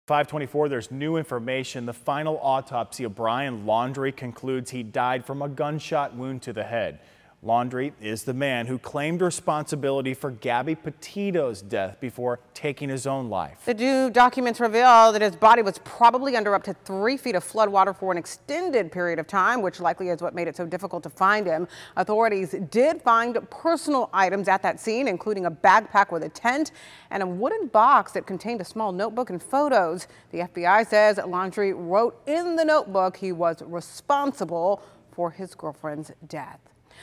Autopsy determines Brian Laundrie died by suicide
An official autopsy report conducted by a Florida medical examiner has determined that Brian Laundrie died by a suicide.